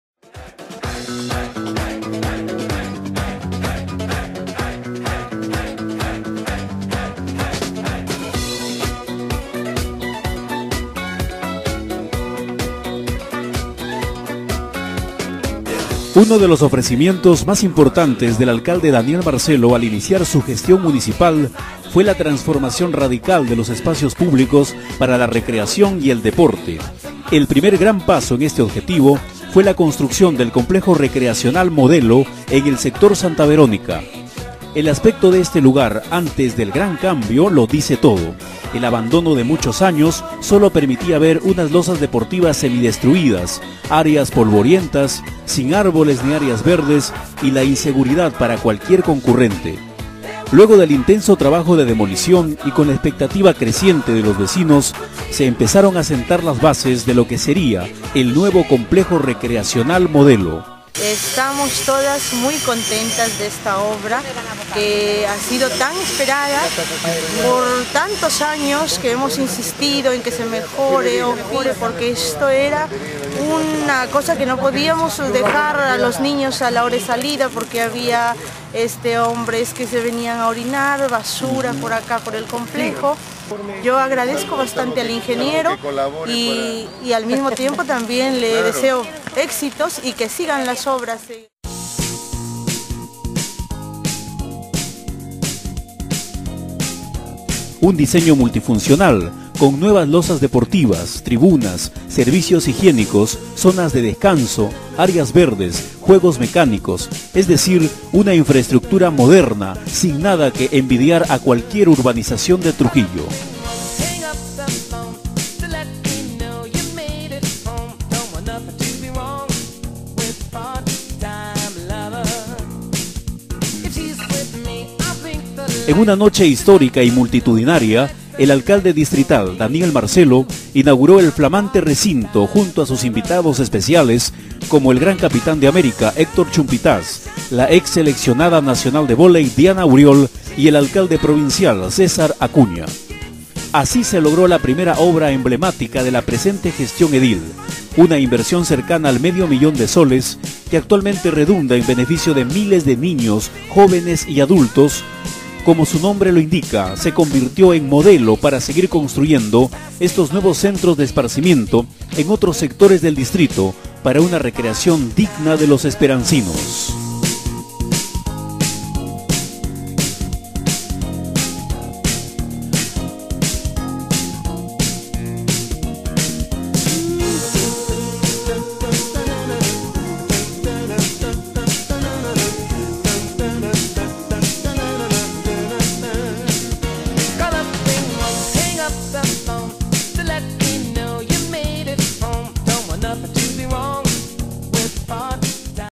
complejo modelo la esperanza
gran complejo deportivo MODELO, ubicado en el sector Sta.Verónica - Distrito La Esperanza - Trujillo
Alcalde - DANIEL MARCELO JACINTO